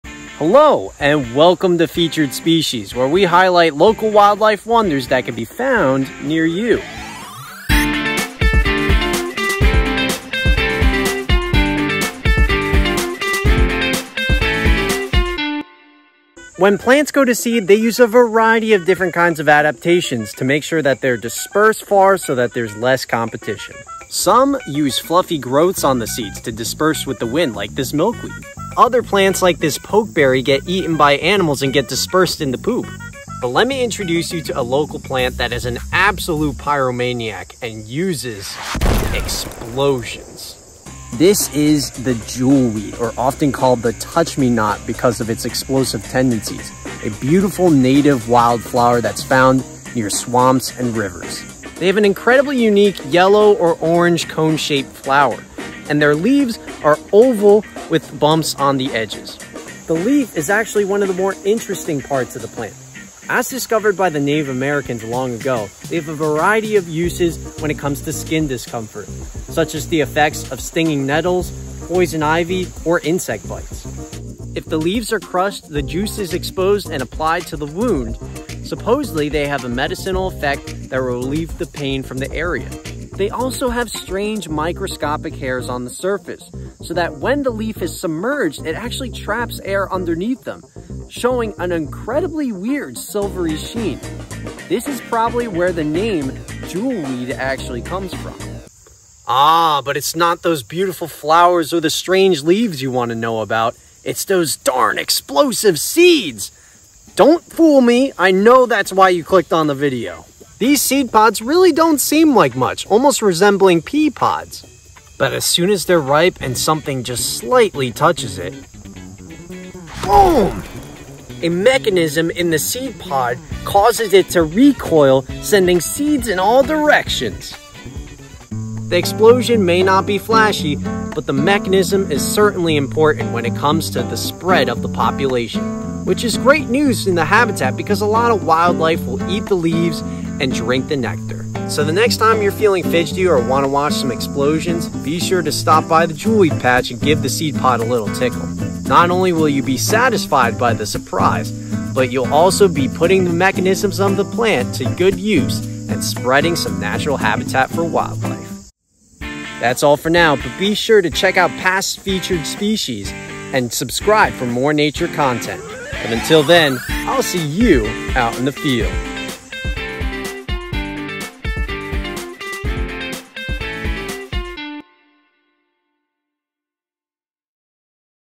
Seed Explosion! Jewelweed - Featured Species
Explosive Seeds? Silvery Leaves? The Jewelweed is filled with strange surprises! Learn about it's secrets in this weeks latest Featured Species.